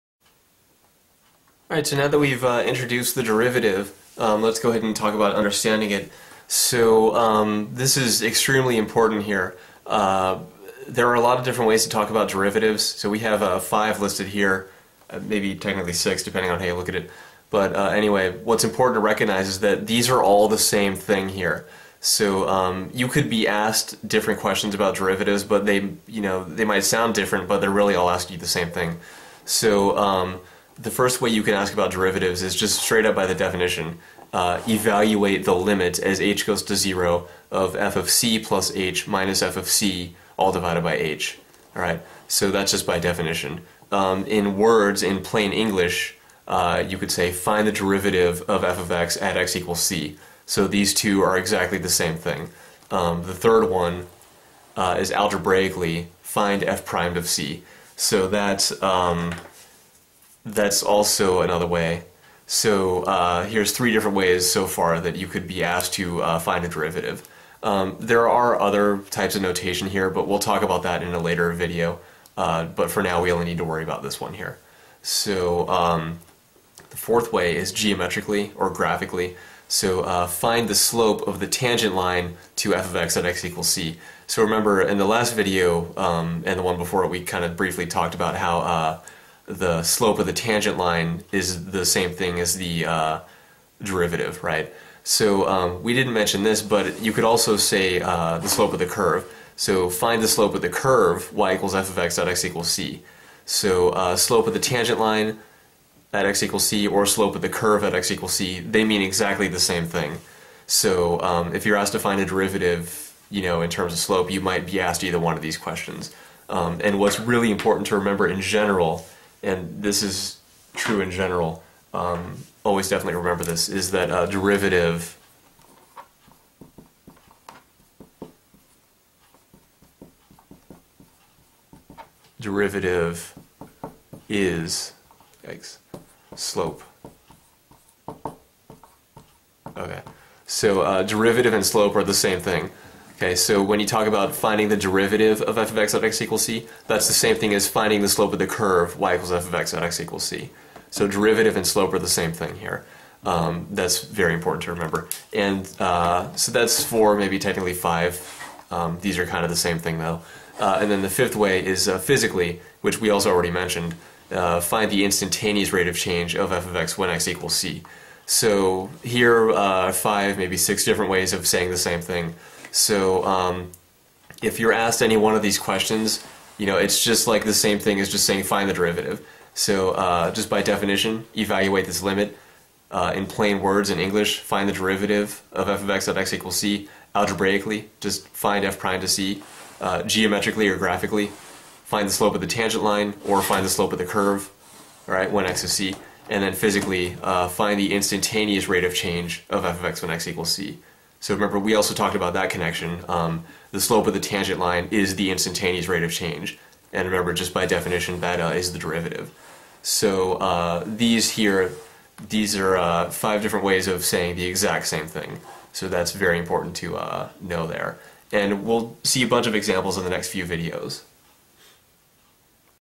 Calculus I - Understanding Derivatives
This video explains the various ways in which derivatives can be interpreted.  Very important stuff!